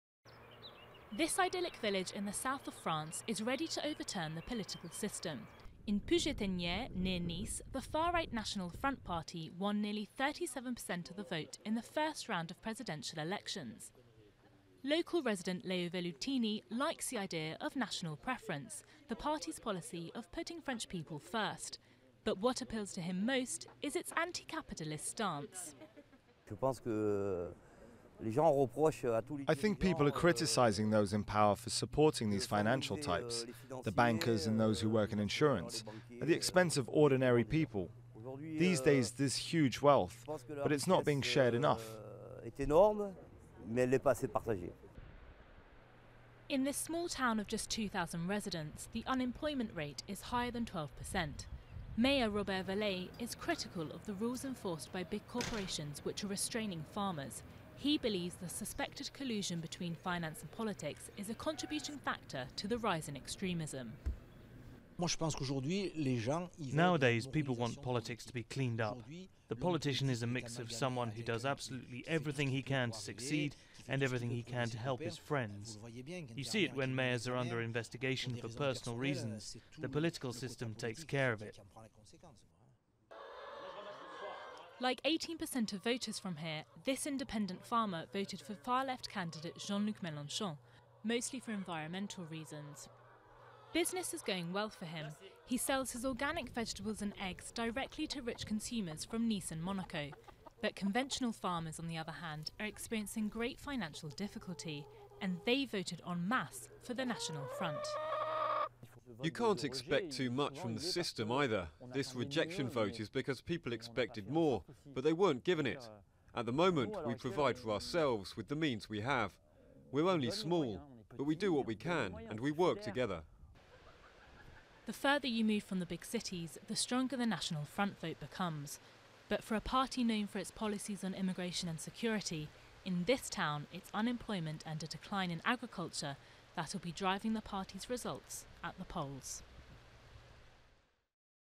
Picture-postcard Provence village turning to the far-right
Puget-Théniers, a picture-postcard village in Provence, is representative of the quiet political rebellion underway in rural areas of France. An idyllic town situated halfway between mountains and the Mediterranean, the locals of this village voted overwhelmingly for 'anti-establishment' candidates in the first round of elections, with 37 per cent going for far-right second-round candidate Marine Le Pen.